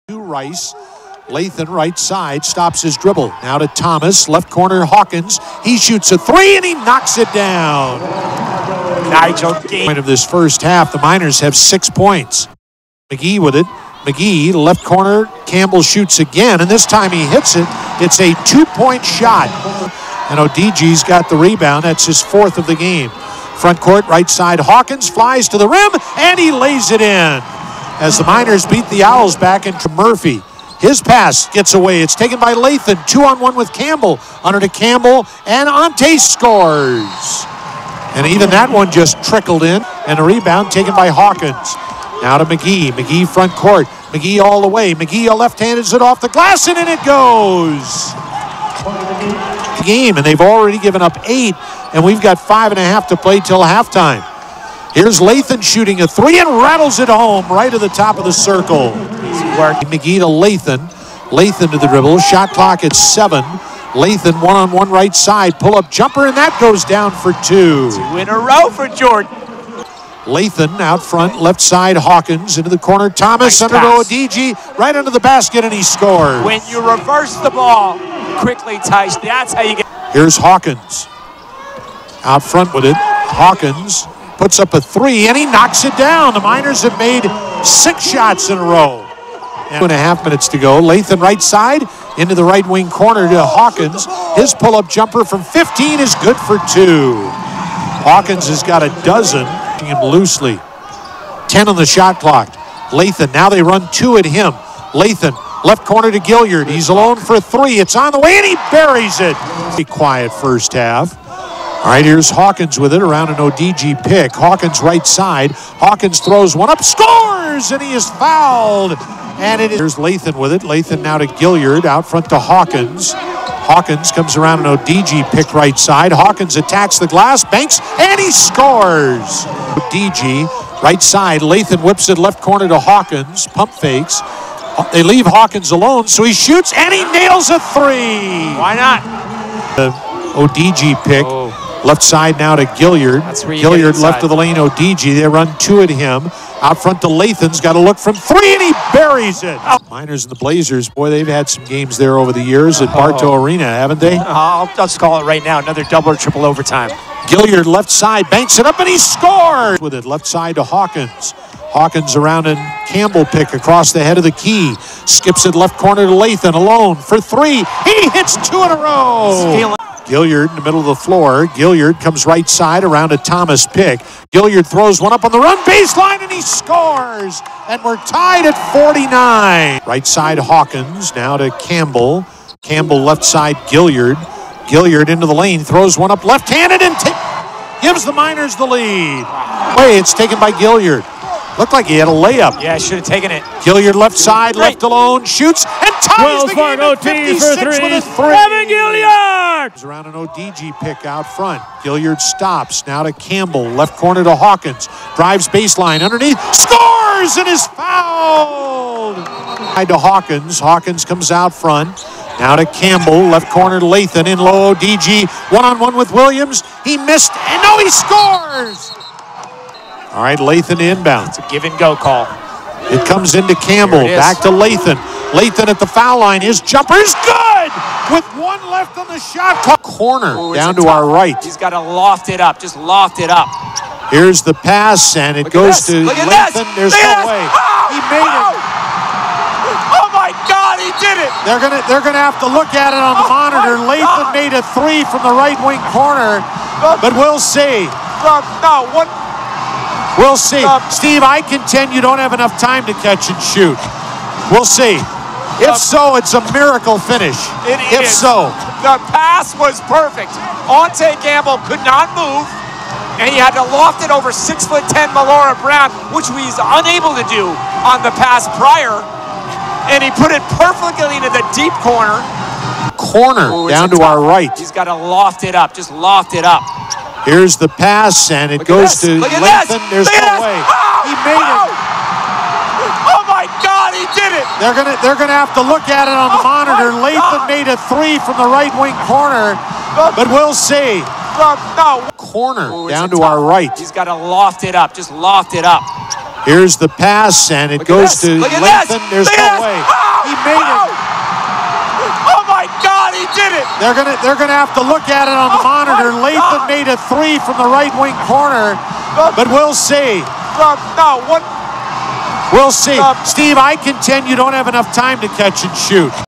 HIGHLIGHTS: UTEP vs Rice (1-12-19)
UTEP beats Rice at the buzzer.